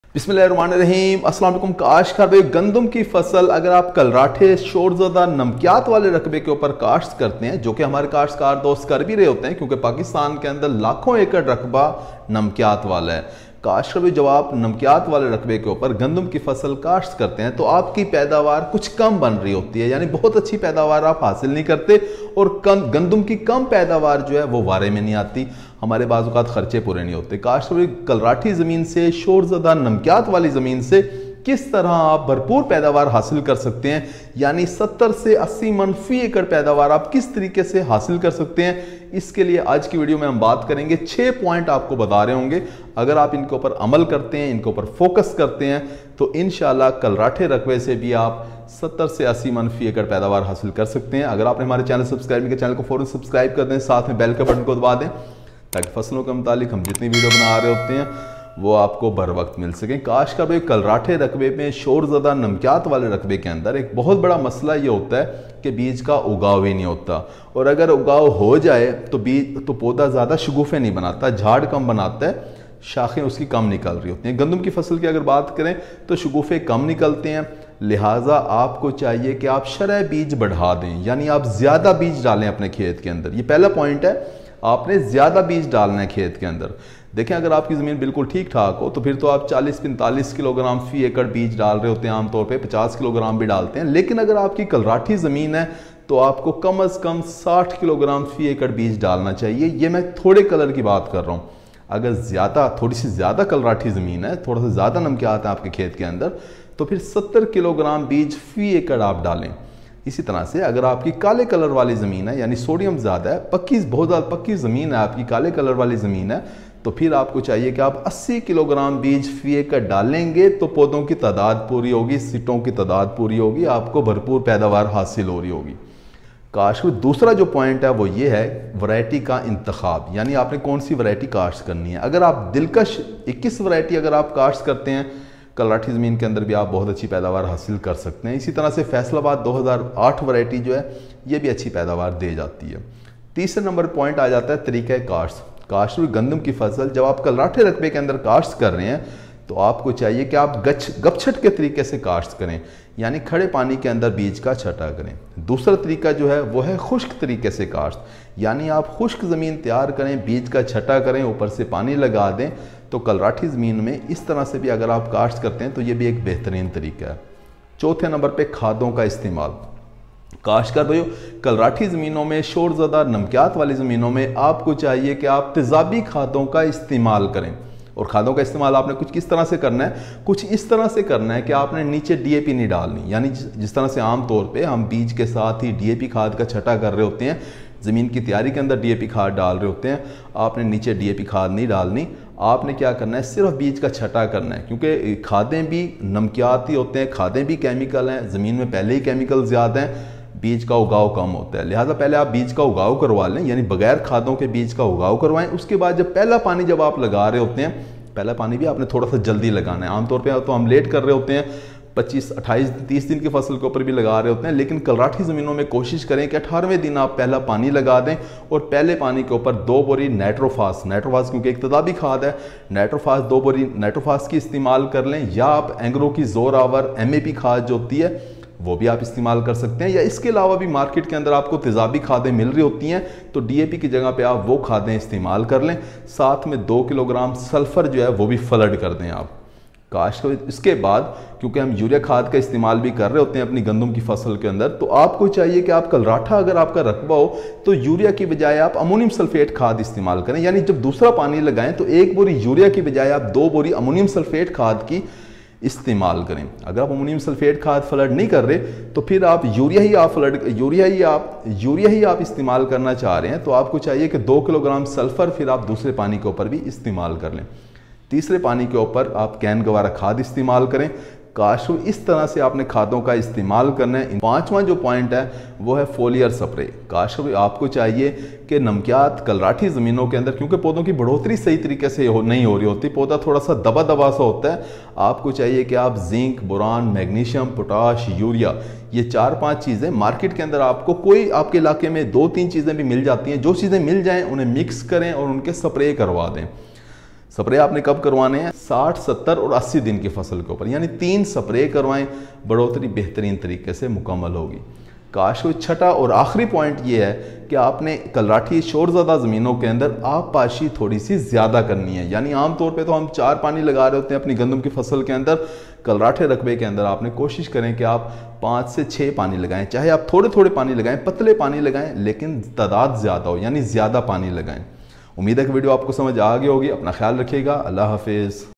How to grow wheat crop in saline soil | Wheat cultivation in salts affected soil | Abid Ali Agrarian
In this video, I will share the tips to grow wheat crop in saline soils.
Wheat crop profitability is usually low in salts affected soils which cause desperation in wheat farming community.
Following are the points which need to focus by our growers to get good yield from wheat crop in salts affected soils;
1) Higher seed rate to increase plant population
2) Gupchhat sowing method
3) Acidic fertiliser usage
4) Frequent irrigations
5) Foliar nutrition
6) Variety selection